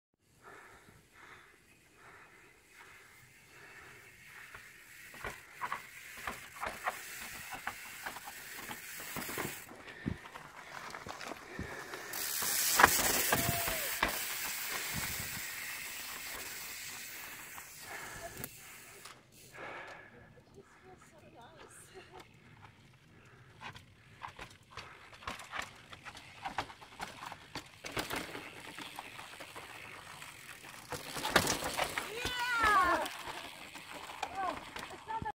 Bad Ass Girls on the zen Drop
Kathleen and Heather dropping the Zen.